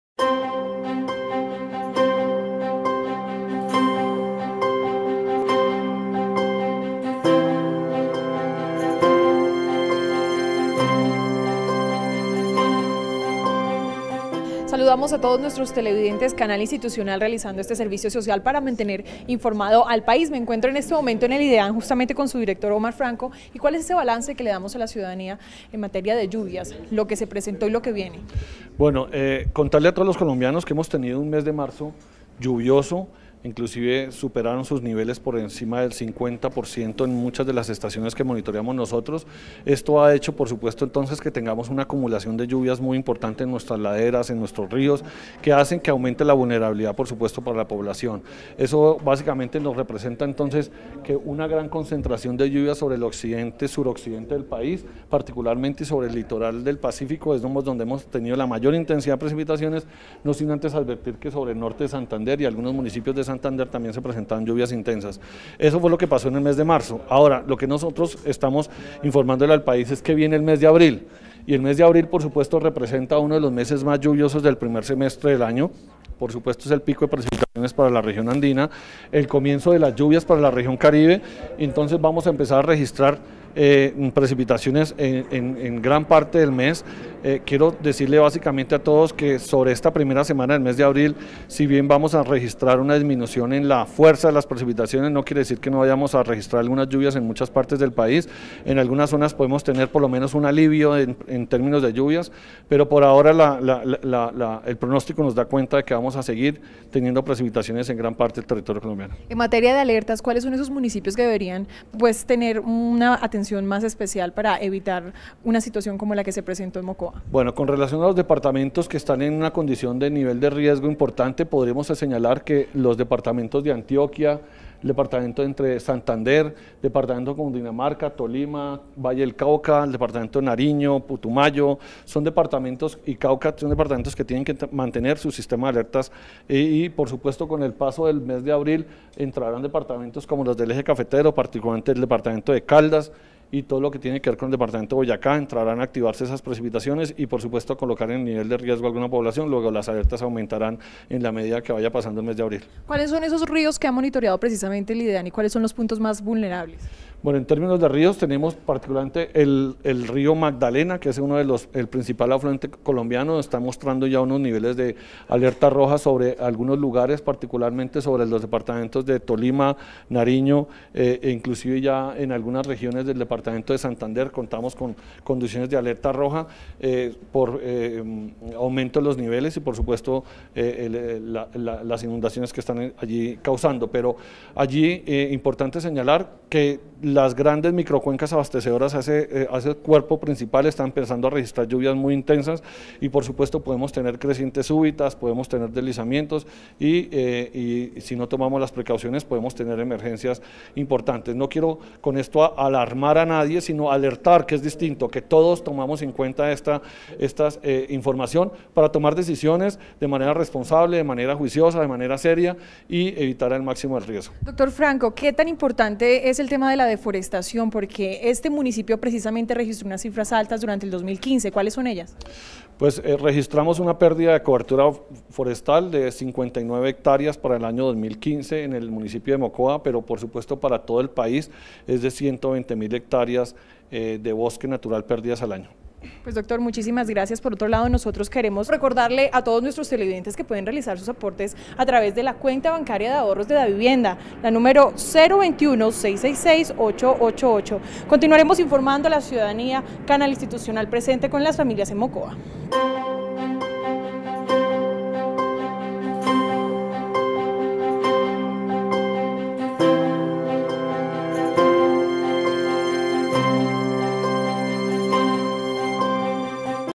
Abril aguas mil
Antioquia, Santander, Tolima, Valle del Cauca, Nariño, Cundinamarca, y Putumayo, deben mantener su sistema de alertas. En abril el eje cafetero y Boyacá.